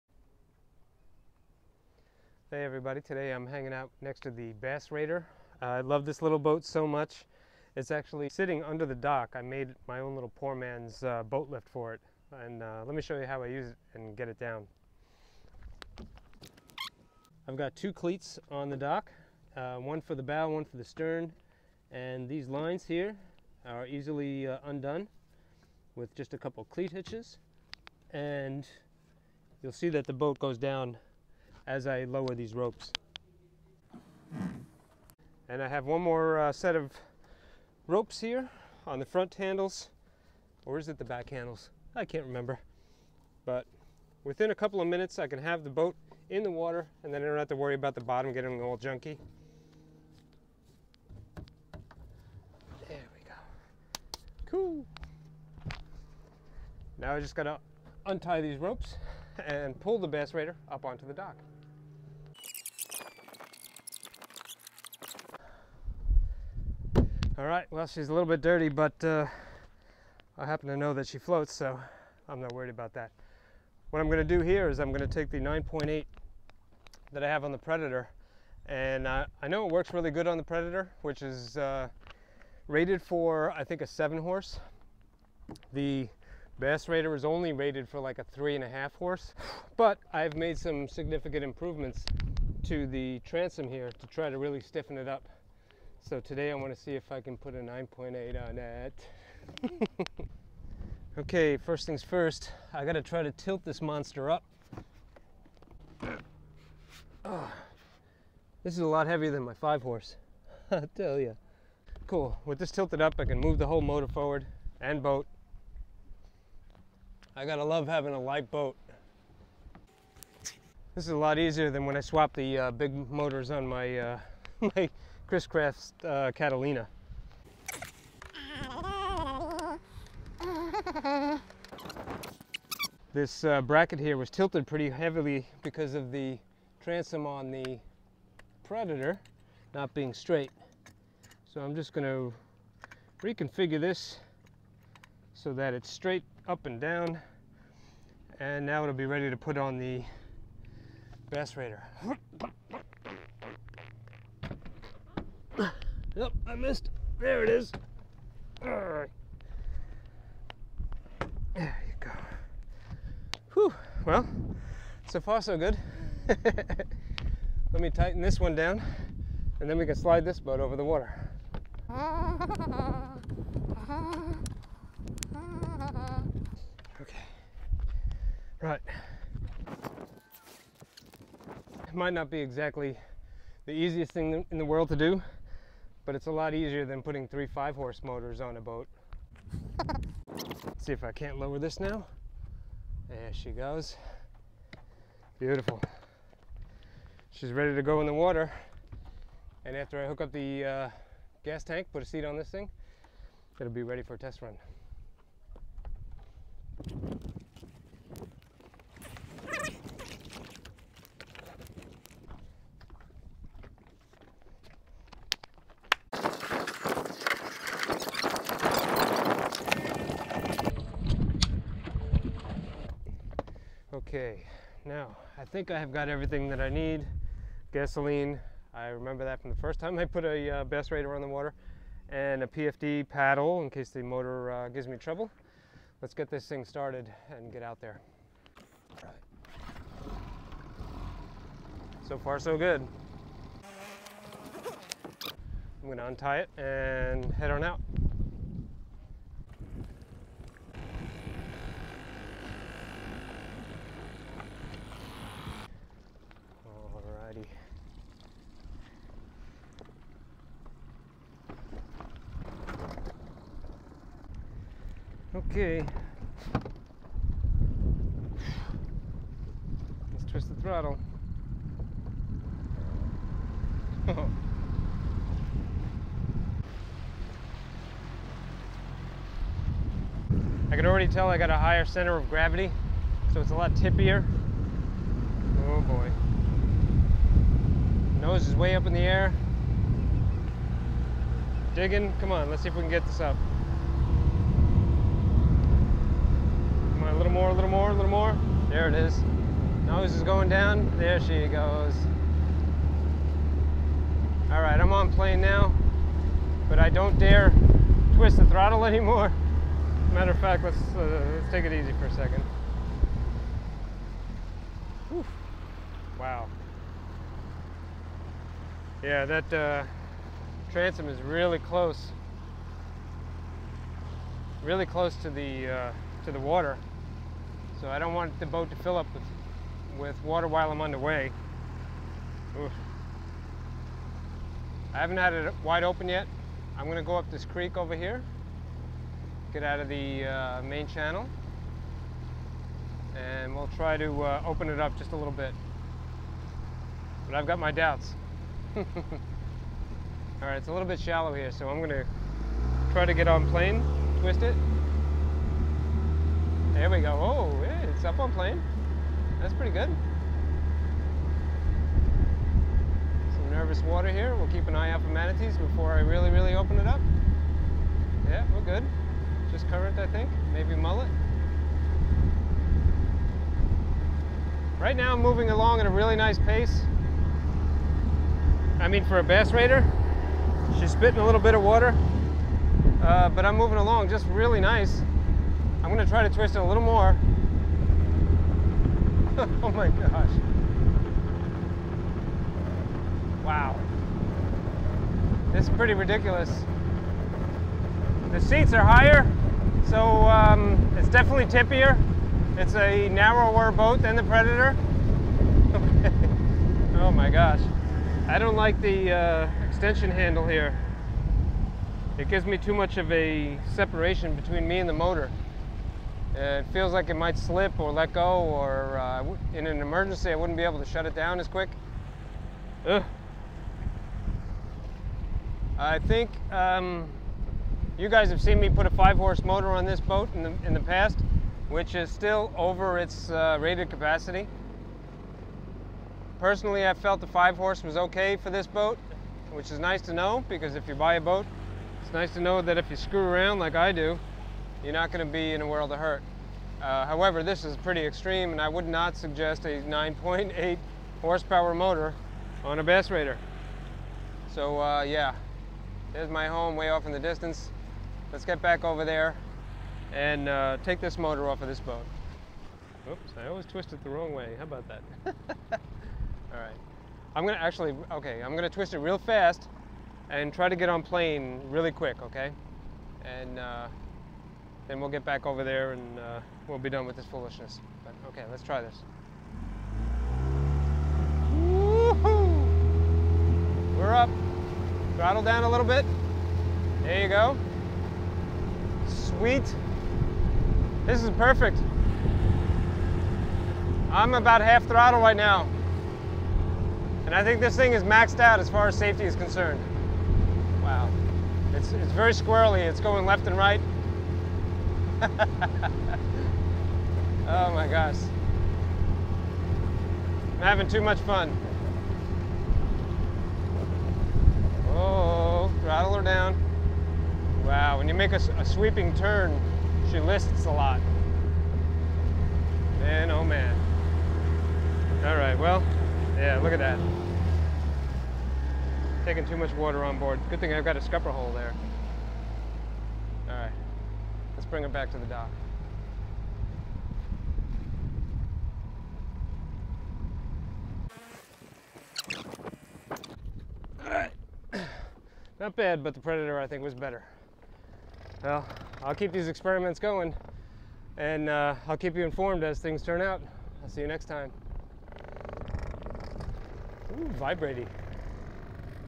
The Life of Marty 020
Can I put a 9.8hp motor on a boat designed for a 3.5? Well, I do! The real question is, 'Should I?' It is a stout, iconic boat that I have successfully overpowered in the past but this time I may have found it's limits.